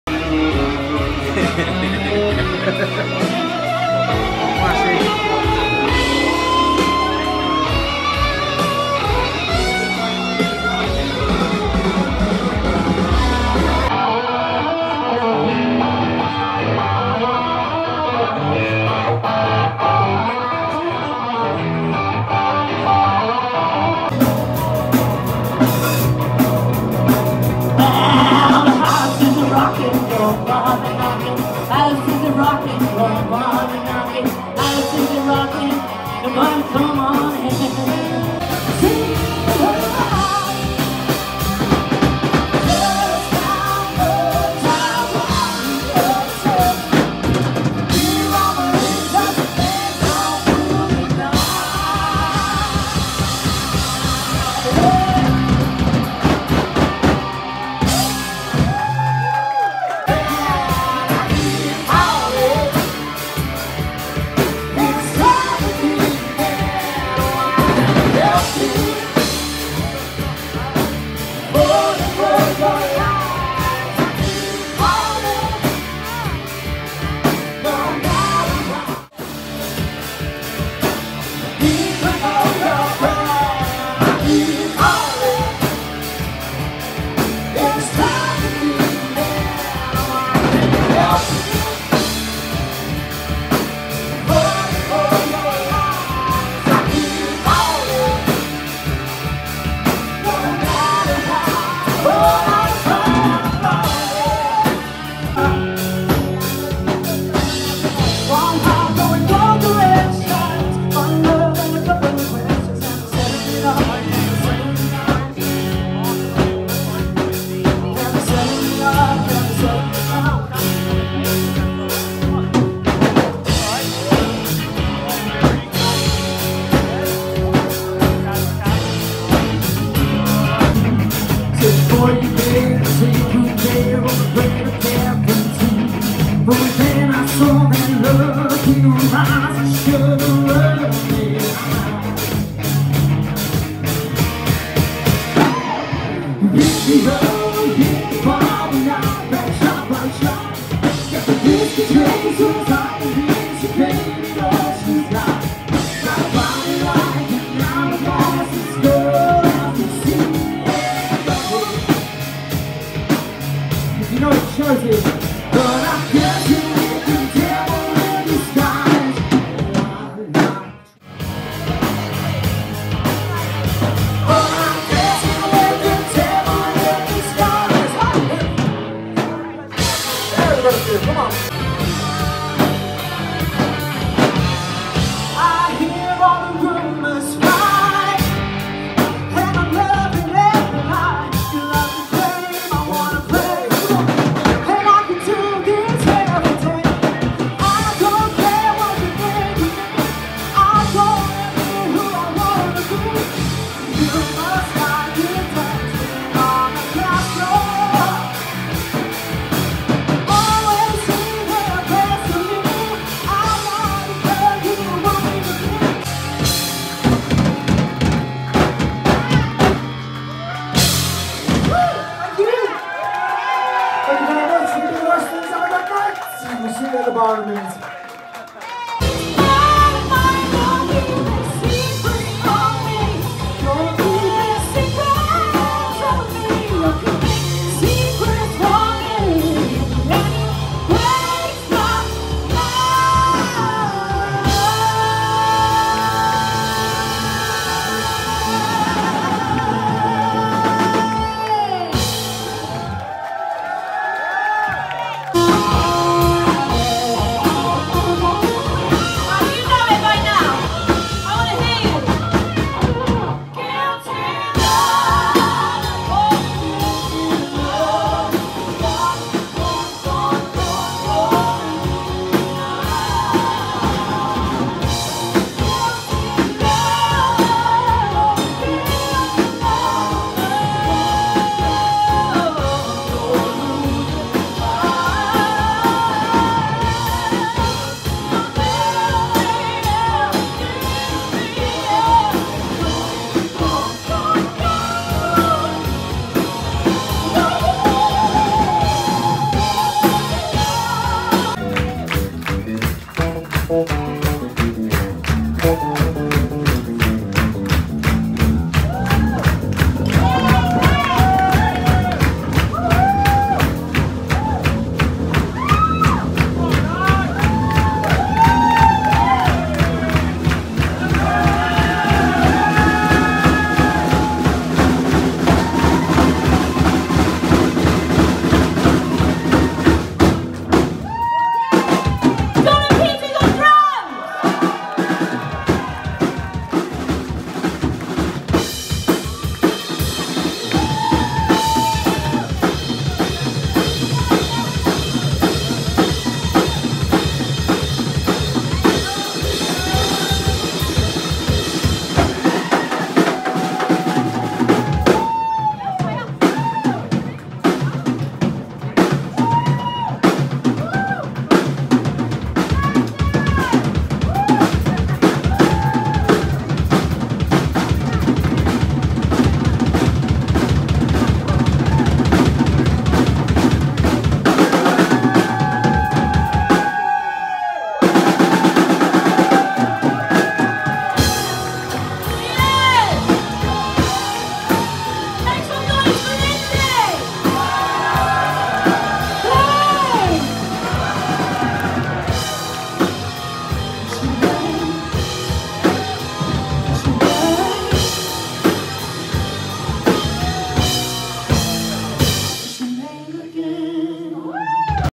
The Westlands Live at The Giffard Arms
What an amazing night we had at The Giffard Arms Playing Only Our 2nd show ever as a full band.

On the night we played with great bands Jane and The Hurricanes  and Headline Band Brave Rival.

A great night of Blues Rock and Country